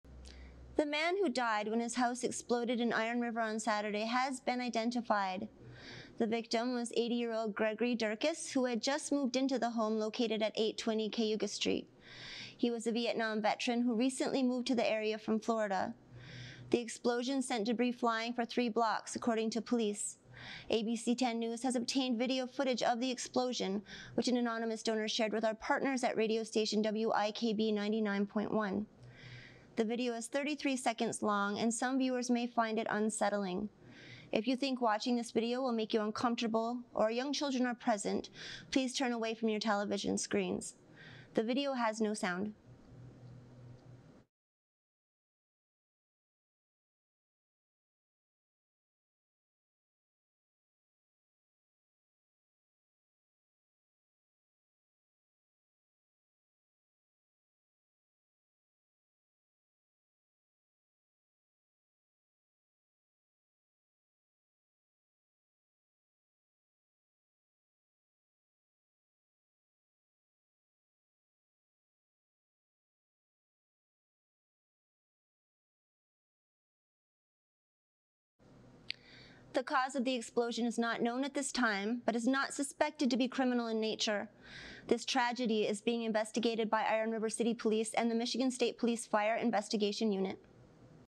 Iron River Explosion Claims Life of Vietnam Veteran
The man who died when his house exploded in Iron River on Saturday has been identified.
The victim was 80 year old Gregory Dierkes who had just moved into the home located at 820 Cayuga street.
He was a Vietnam veteran who recently moved to the area from Florida.
The explosion sent debris flying for three blocks according to police.
ABC 10 News has obtained video footage of the explosion which an anonymous donor shared with our partners at radio station WIKB 99.1.
The video is 33 seconds long and some viewers may find it unsettling.